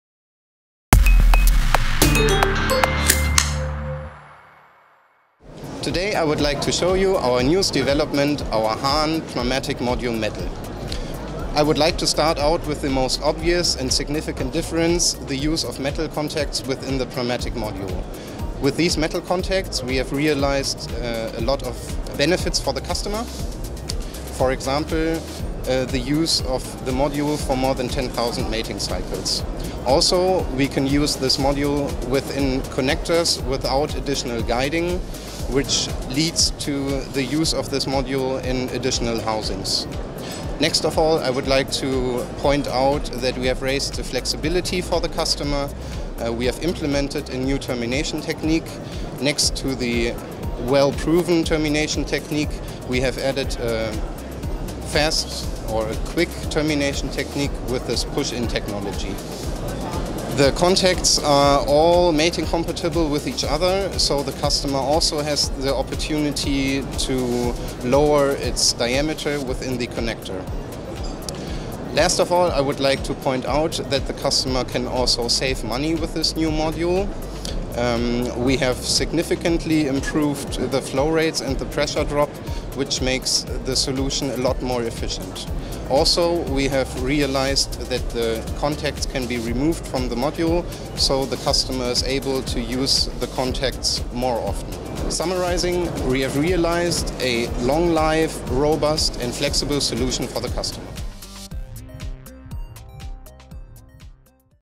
Han® Pneumatic Module Metal
High mating cycles - For compressed air distribution systems

Ever increasing demands are being made on industrial production interfaces. Networked facilities require greater flexibility than ever before. Production facilities requiring frequent conversion need connectors that can be plugged and unplugged more frequently.

The new Han® Pneumatic Module Metal from HARTING enables frequent mating cycles for compressed air distribution systems, too. Pneumatic applications , e.g. for supply to transmission units, controls, holding and unloading units are gaining increasing levels of flexibility.

HARTING uses superior quality High Mating Cycle contacts made of metal to achieve a high number of mating cycles. Whereas plastic contacts can only be plugged and unplugged 500 times, over 10,000 mating cycles have now been made possible.

The new interface now also works at a higher operating pressure: instead of the previous upper limit of 8 bars (Han® Pneumatic-Module with plastic contacts) the new metal contacts can withstand 10 bars of pressure - permanently. This means that the Han® Pneumatic Module Metal can handle the entire low-pressure range. There are now many new application options for mateable compressed air distribution systems.

The use of metal contacts also opens up new combination possibilities for pneumatics in connectors for high mating cycles (HMC): electrical contacts for the transfer of signal and power can now be combined with contacts for the transfer of compressed air in HMC connectors for the first time – saving users valuable build space and assembly time.